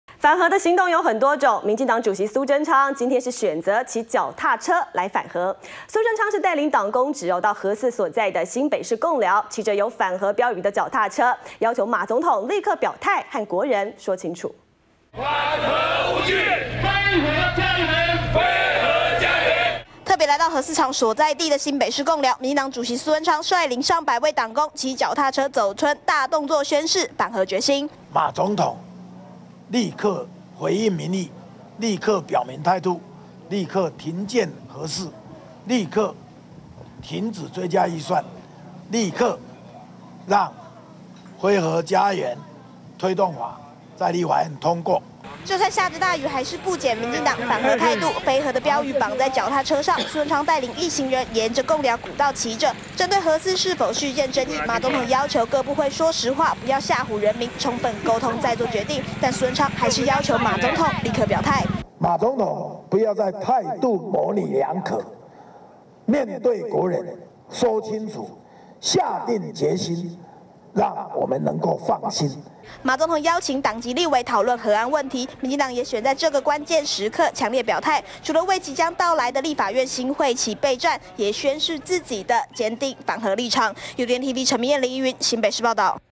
【2013.02.23】核四續建爭議 蘇貞昌籲馬表態 -udn tv
反核行動有很多種，民進黨主席蘇貞昌今天就選擇騎腳踏車反核，蘇貞昌帶領黨工職到核四所在的新北市貢寮，騎著有反核標語的腳踏車，要求馬總統立刻表態和國人說清楚。